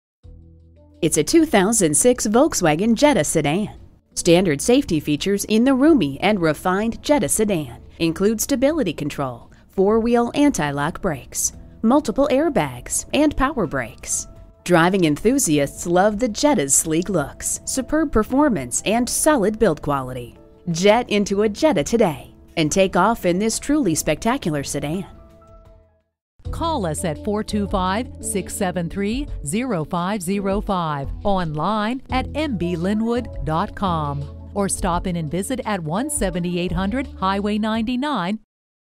2006 Volkswagen Jetta Lynnwood WA Seattle, WA 25182A - SOLD
Year: 2006
Make: Volkswagen
Model: Jetta
Trim: Manual
Engine: 5 Cylinder
Transmission: Manual
Color: Gray
Interior: Anthracite
Mileage: 52261
Stock #: 25182A
VIN: 3VWPF71K76M795847

Address:
17800 Highway 99